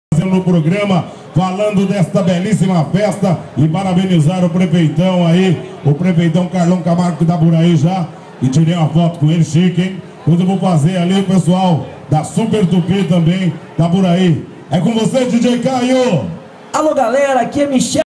220913 = 5° Queima do Alho Granja Viana - Cotia SP = Show
Evento: 220913

5° Queima do Alho Granja Viana - Cotia SP

Adauto Francisco
(Super Radio Tupi - Programa e Revista Conexão Sertaneja)

Produção:
Tarcizio Junior (Programa Conexão Sertaneja);
Nina Santos(Programa Conexão Sertaneja);